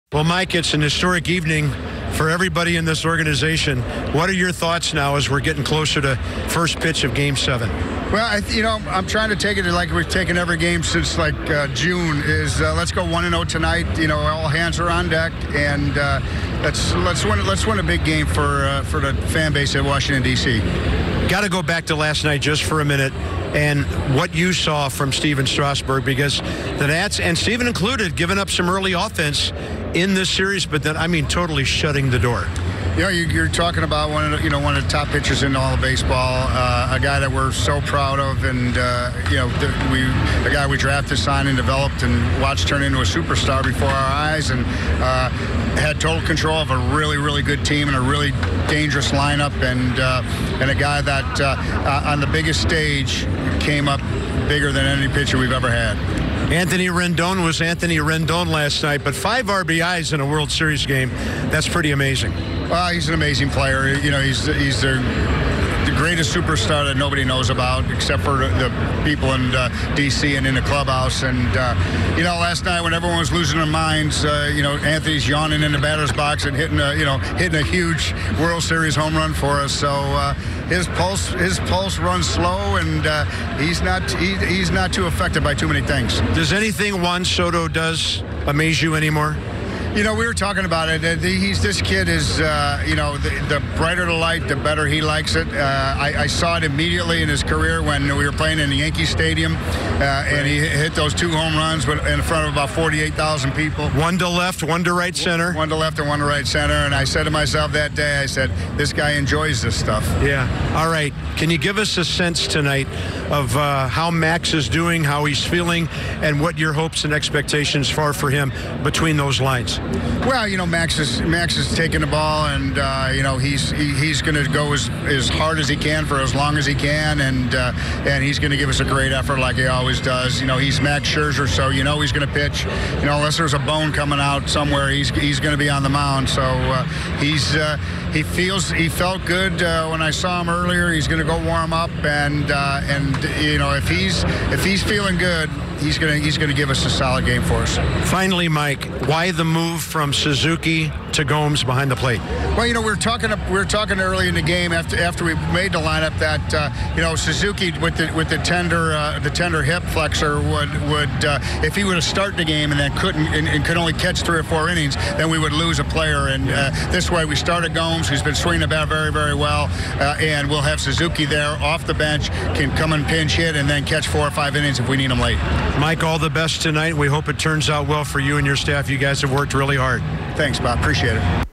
Mike Rizzo prior to World Series Game 7
Bob Carpenter gets Nationals' GM Mike Rizzo's thoughts prior to Game 7 of the 2019 World Series. More